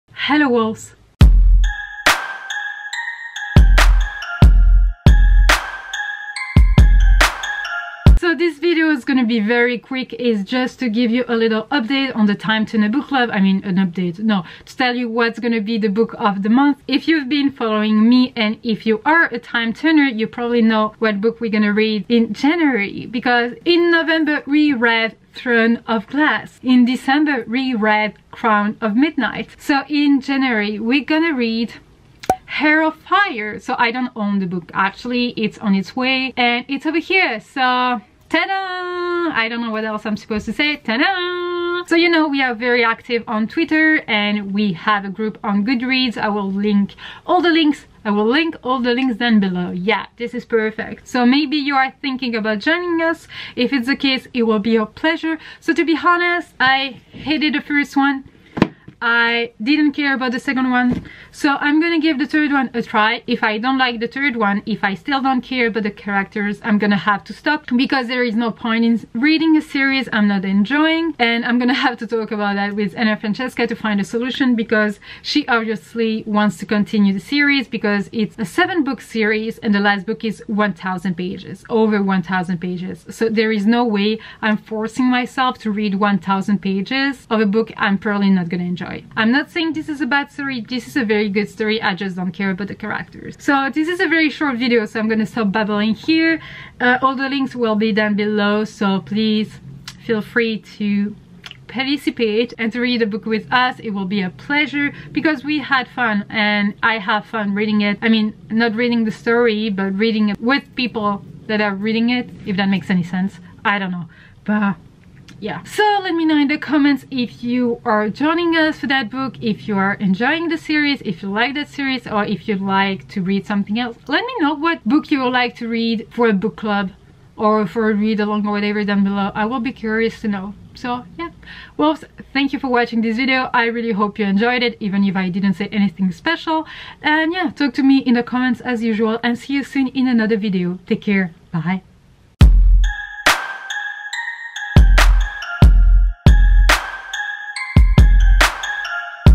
TIME TURNER BOOK CLUB | Announcement | January 2019
Open Me! I won't bite you ha ha

Hello Wolves,

What's up?

I hope you enjoyed the announcement for the Time Tuner Book Club and that you will join us. I really really hope I will start enjoying that series with that book...

And if you did read what I just wrote, add the word "Fae" in your comment.

Time Turner Book Club:

IG hashtag

Artiste : __url_artiste__

The Music I fell in love with:
Musique proposée par La Musique Libre